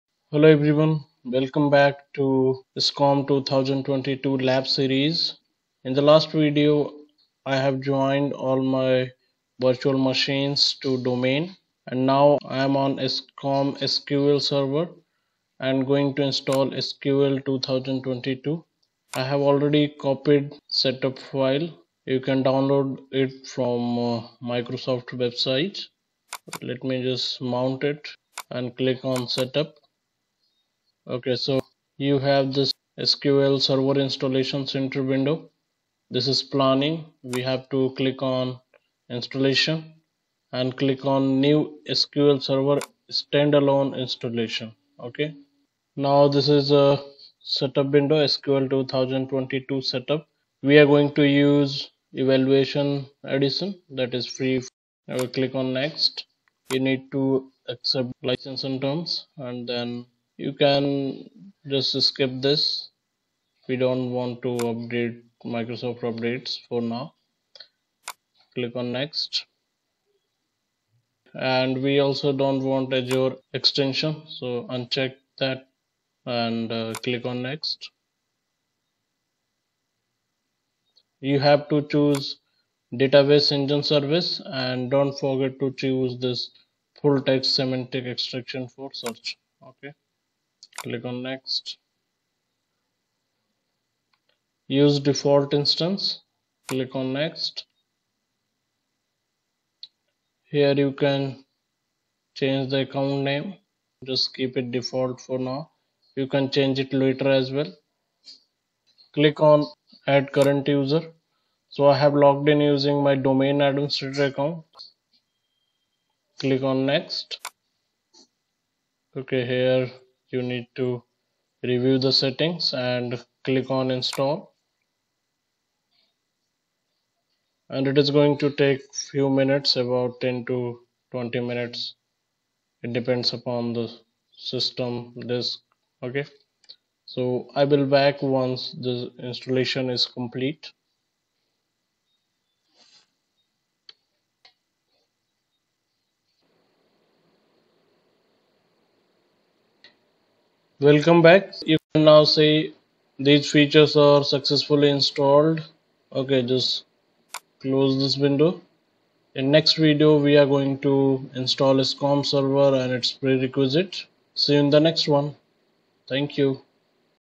SCOM 2022 Administrator : SQL 2022 Installation
This video shows installation of SQL 2022 for SCOM server